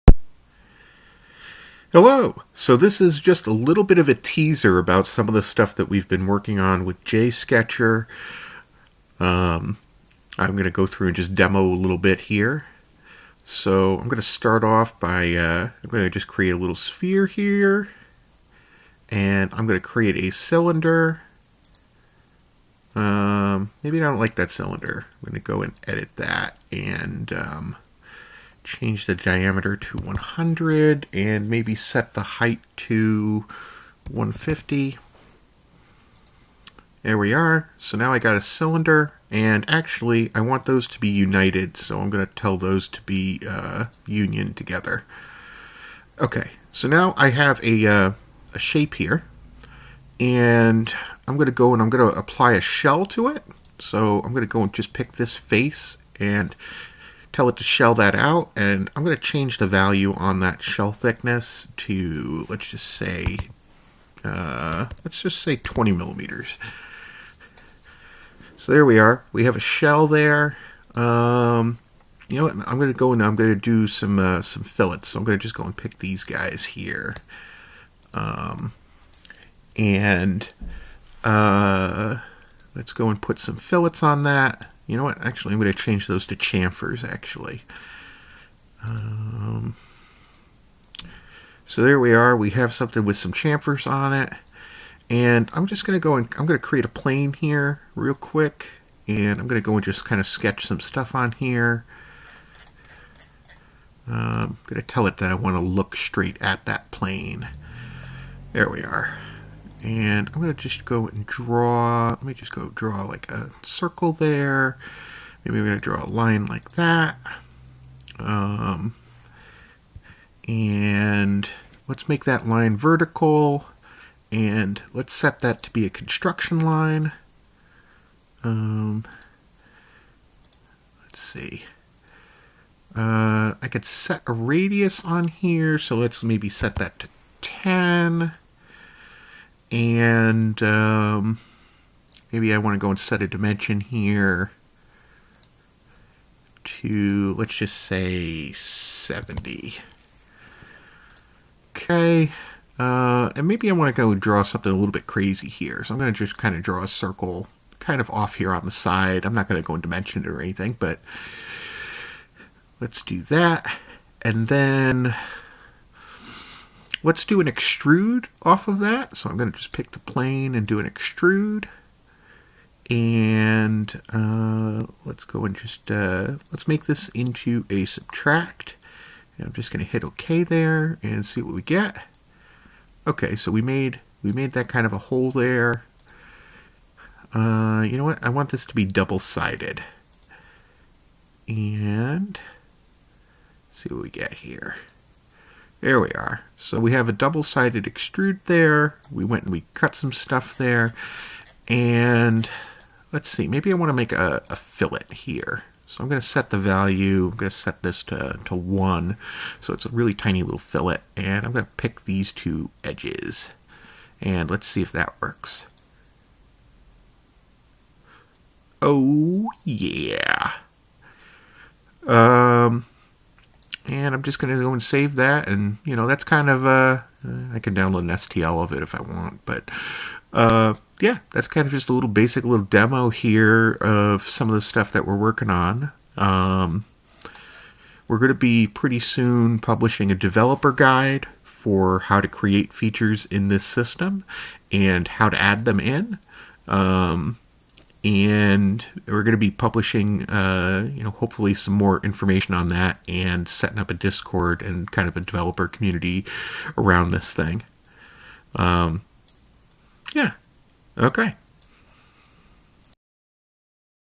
Jsketcher updates teaser. Browser based parametric feature history/constraint based sketches modeler
Mouse position in the screen recording was off positions. Don't know why but it was recorded on a chrome book. Jsketcher is a browser based parametric feature based 3d modeling tool that runs in the browser. This is a video showing an in work development build using a fresh openCascade based wasm for the BREP engine and integrating the 2d sketcher and feature history pipeline. New feature commands are defined using reuable widgets to build each of the UI components and handled in a structured way that should allow other developers to contribute to the project.

Next teaser video might show some assembly constraints :-D but that depends on how long it takes to polish the core modeler code.
An overview of the project can be seen and code is regularly being updated on github.